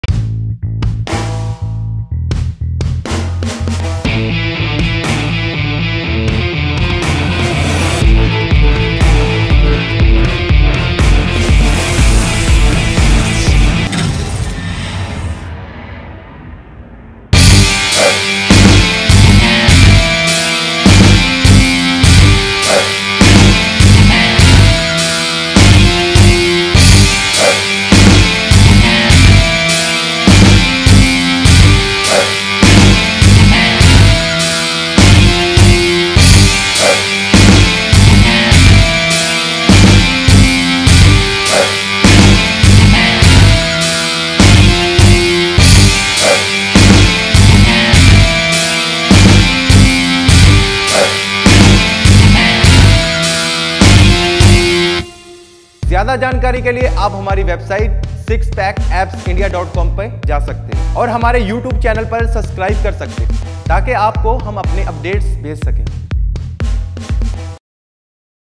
Peacher Curl | Six Pack Biceps India | sixpackabsindia.com
Sixpackabsindia.com body transformation day 1 watch as we transform a fat body with lots of belly fat into a fit and slim body with normal exercise and normal work out .its no magic just normal workout dedication and regular diet